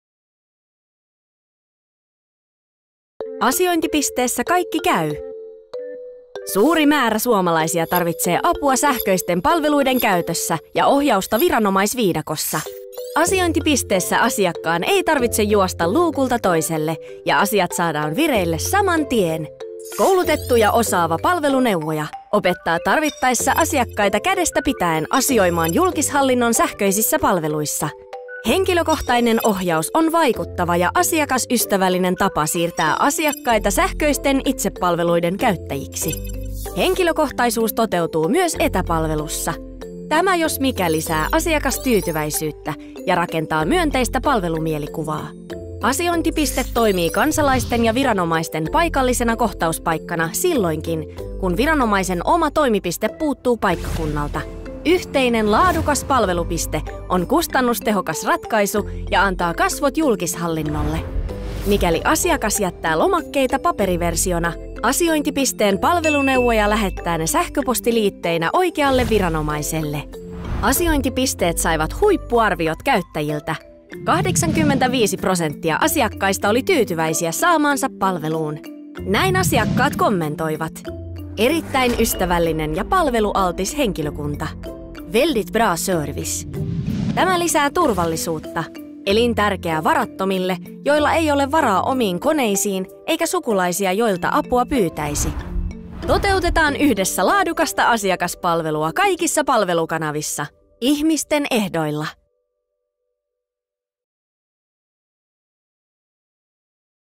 Palveluntuottajien video: Asiointipisteessä kaikki käy!
Asiointipisteessä voit hoitaa monet viranomaisasiat yhden luukun periaatteella.